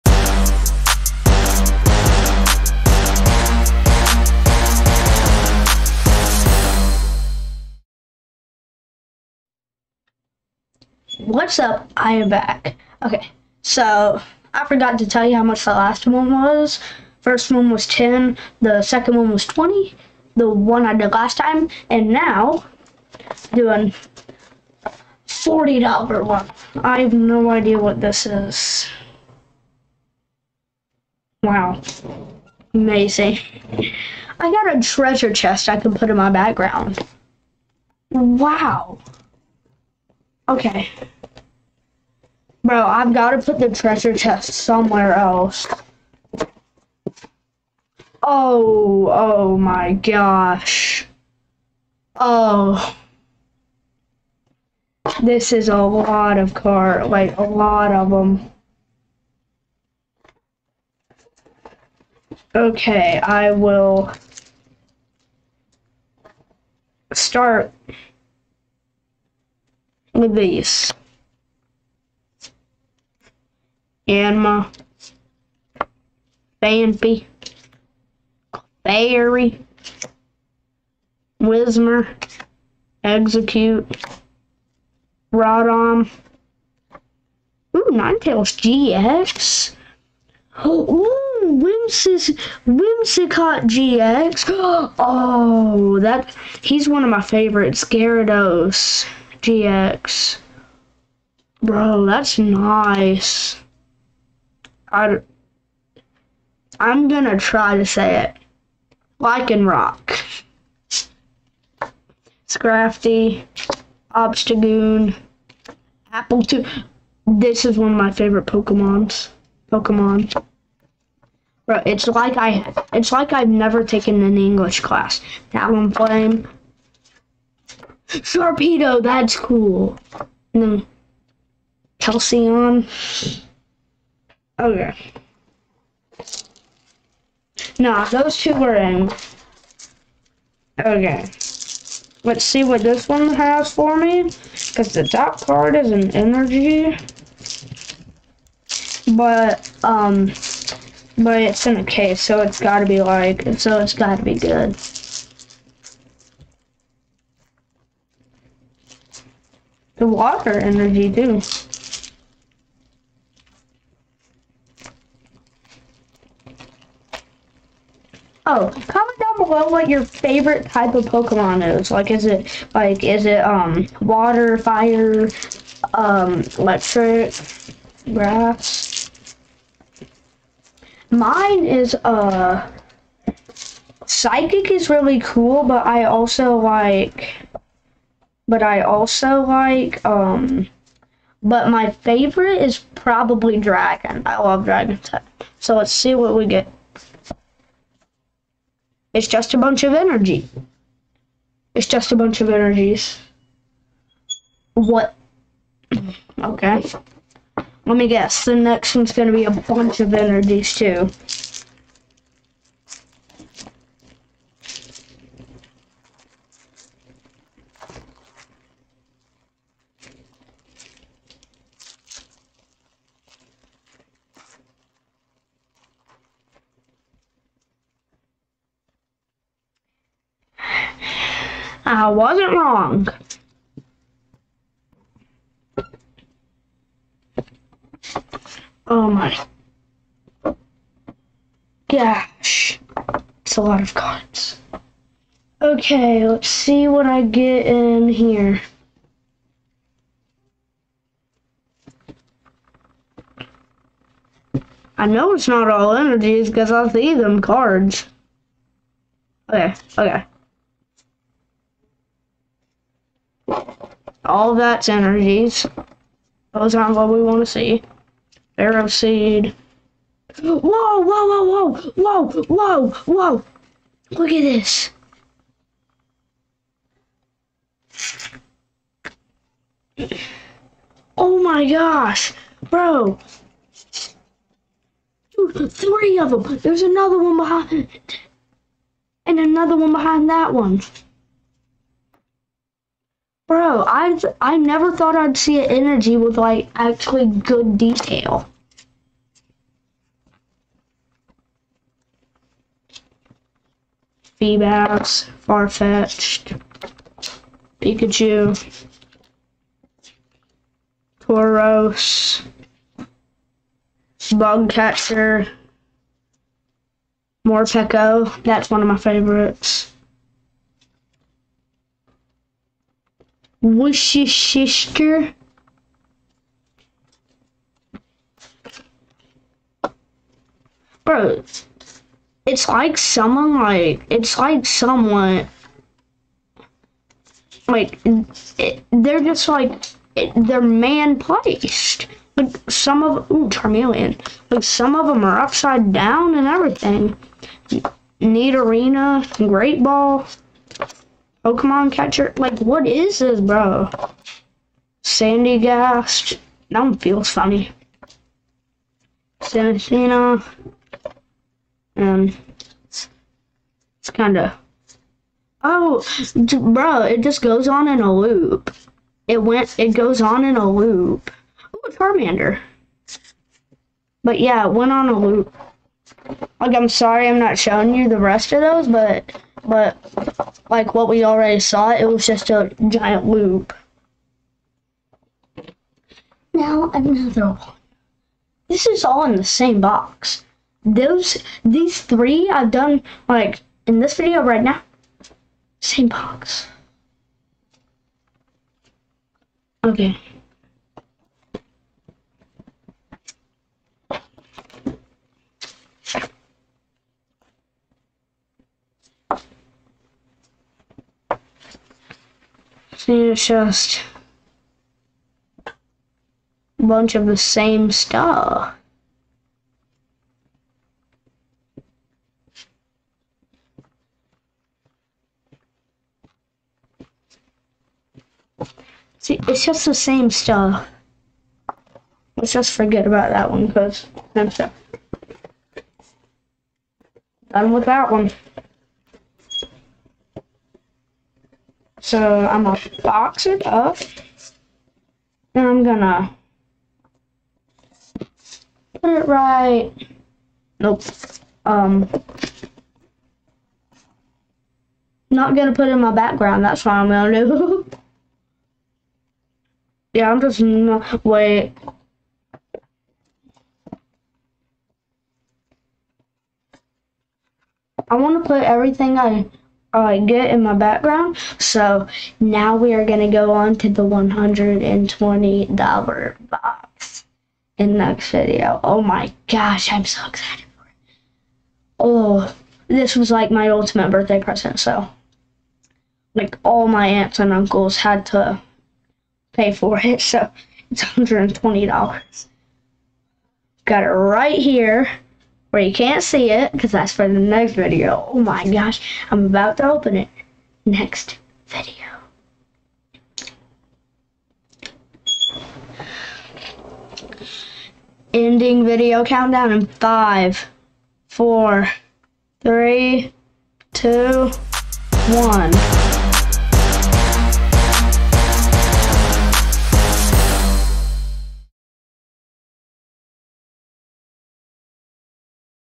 $40 Pokemon Card Unboxing!!! Is it worth it!?!
What's up guys, today we are opening and $40 box of Pokemon Cards.  Our goal is to hopefully get a holographic card.